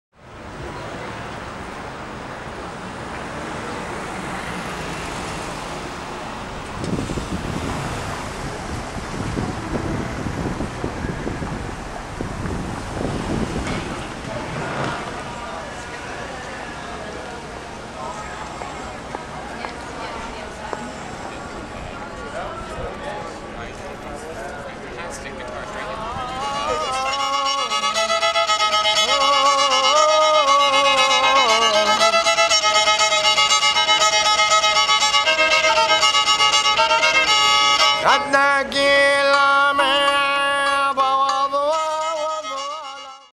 VIDEO: Street scenes during Big Ears
Scenes from the street taken during the Big Ears music festival.